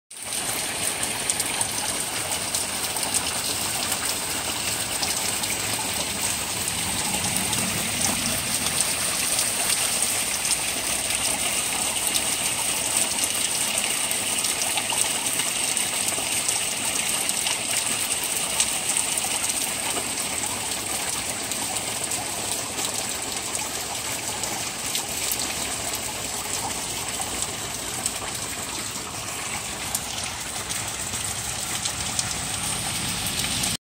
Texas Rain 5-29-2017
Rain at the end of May in Texas! So peaceful!